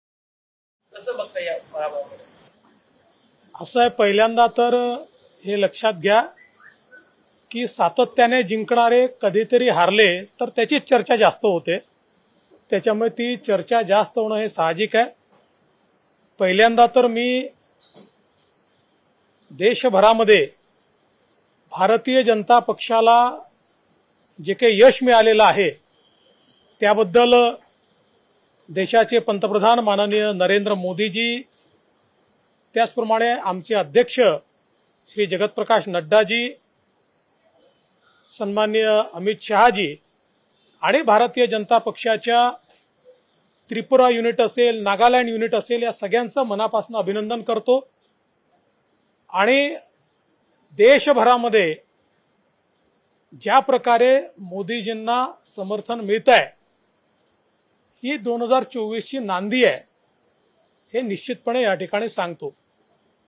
मा.मोदीजींच्या नेतृत्वात ईशान्य भारतात भाजपाचा ऐतिहासिक विजय| मुंबई | DCM Devendra Fadnavis
मा.मोदीजींच्या नेतृत्वात ईशान्य भारतात भाजपाचा ऐतिहासिक विजय| मुंबई | DCM Devendra Fadnavis |  2 मार्च 2023